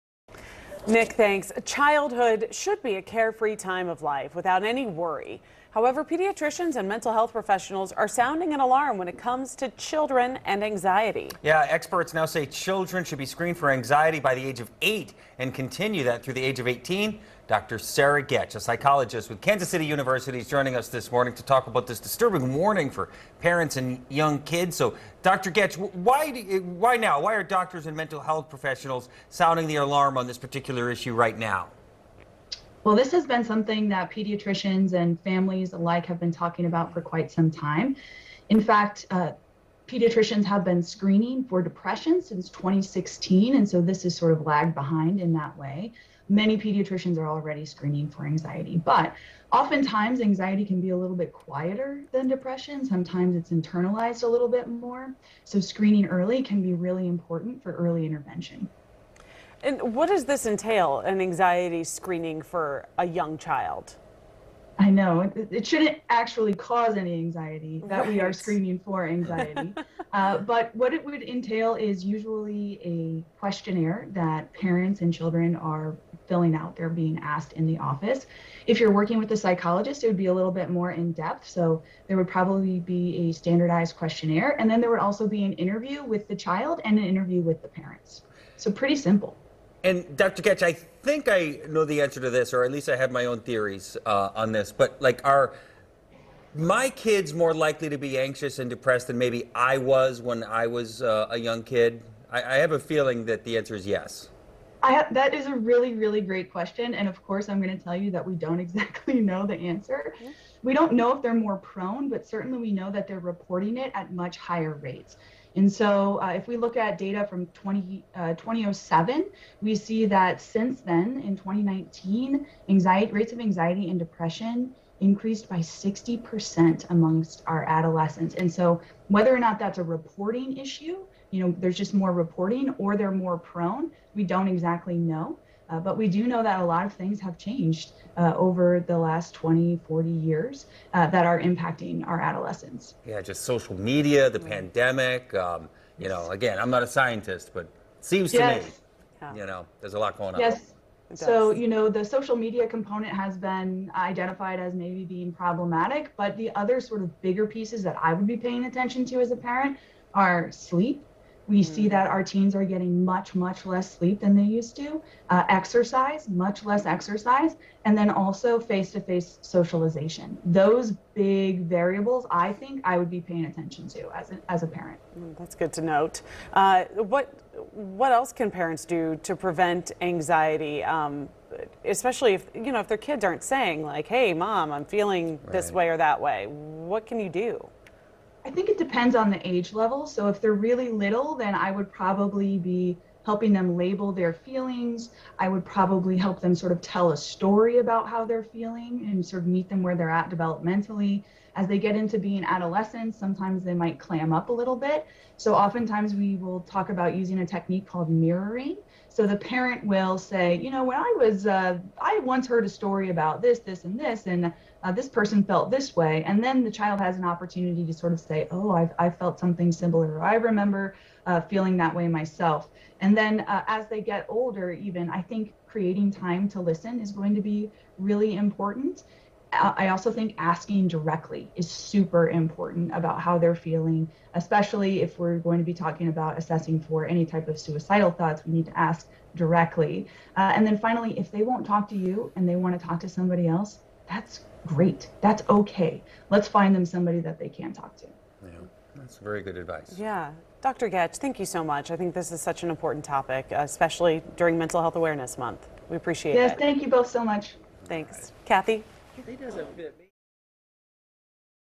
Dr. Sarah Getch - Anxiety Screening for Kids
Dr. Sarah Getch is the featured expert on the WDAF Fox 4 morning show on May 5, 2022